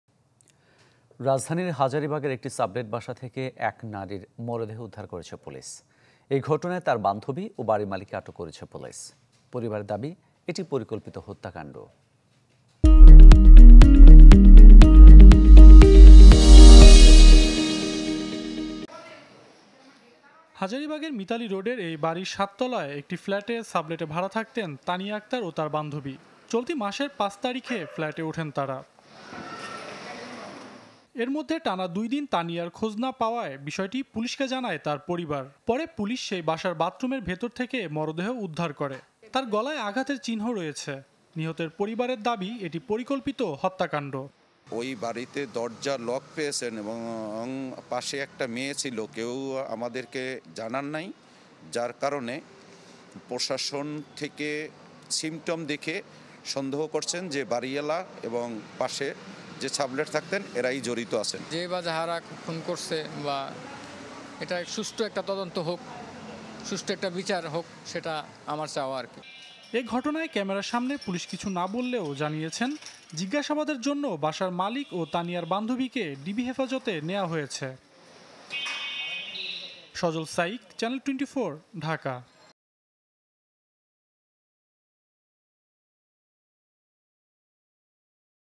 সাবলেট বাসা থেকে নারীর ম র দেহ উদ্ধার; বান্ধবী ও বাড়ির মালিককে আটক | Hazaribag | Dhaka | Channel 24
রাজধানীর হাজারীবাগের একটি সাবলেট বাসা থেকে এক নারীর ম র দেহ উদ্ধার করেছে পুলিশ। এ ঘটনায় তার বান্ধবী ও বাড়ির মালিককে আটক করেছে পুলিশ। পরিবারের দাবি, এটি পরিকল্পিত হ ত্যা কাণ্ড।

»» Office Address:
Bangladesh.